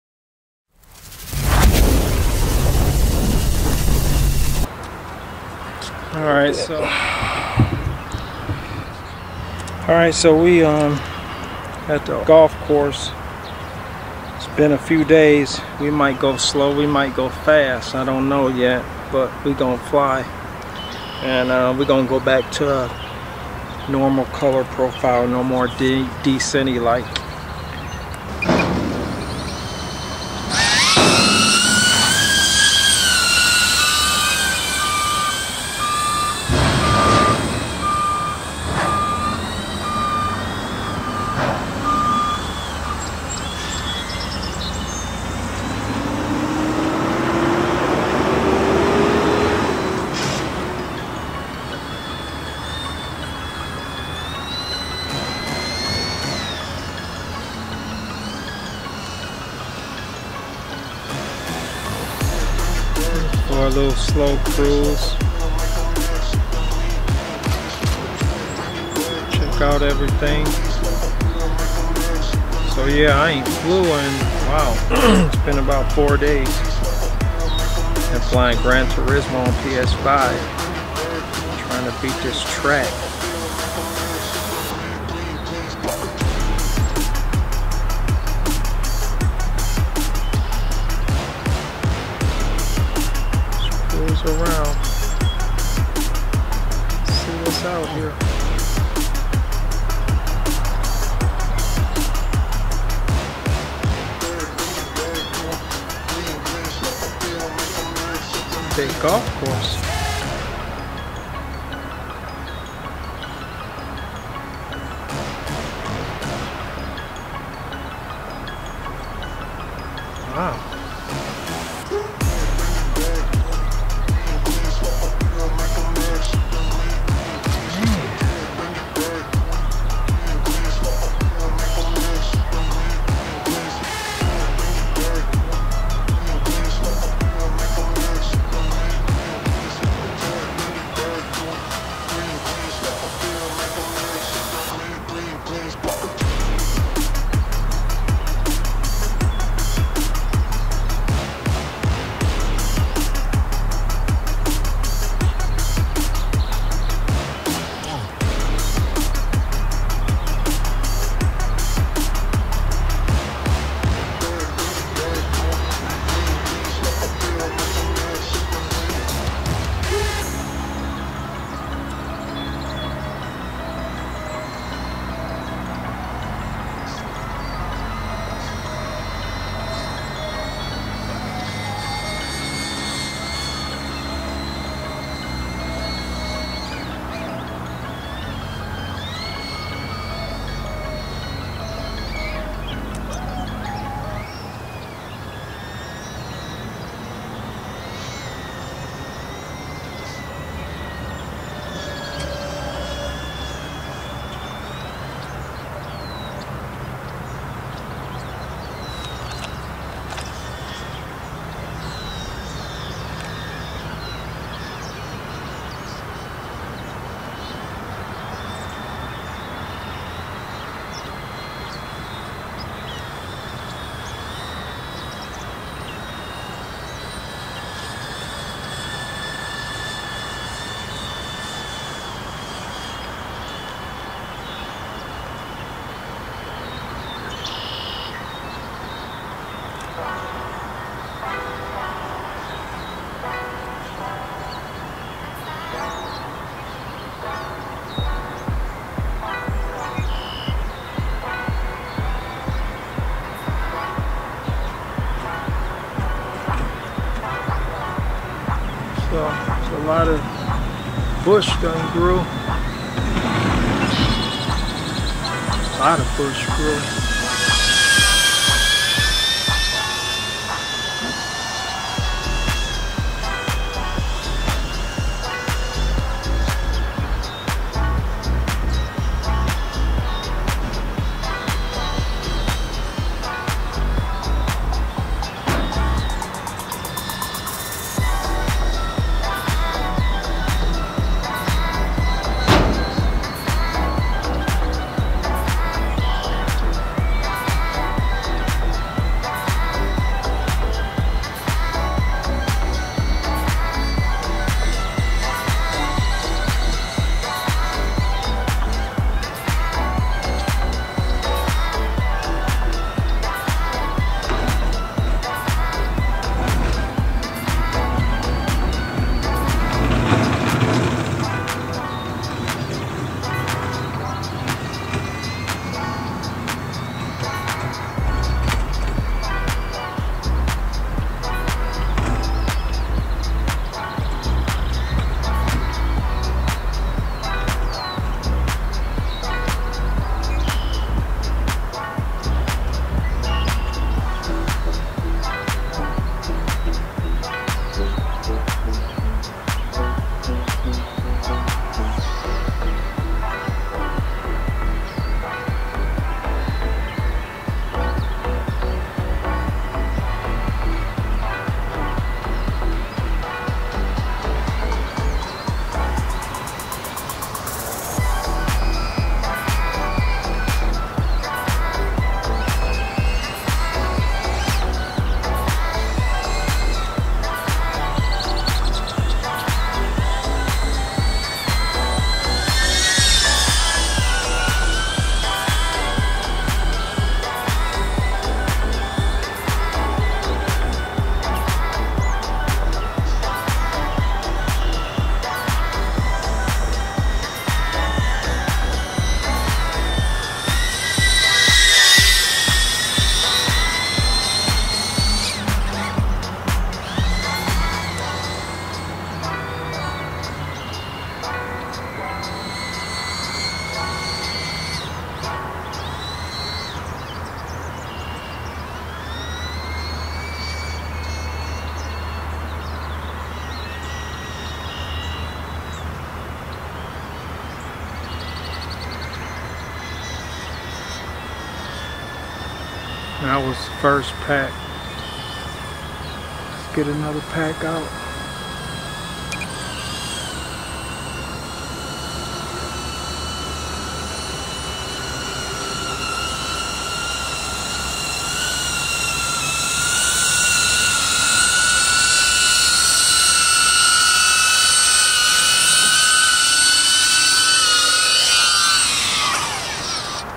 DJI AVATA FREESTYLE FLIGHT VLOG
welcome back everybody its been 5 days since i last flown the dji avata im rusty today but i wanted to practice some freestyle flying to see the limitations the avata has to offer enjoy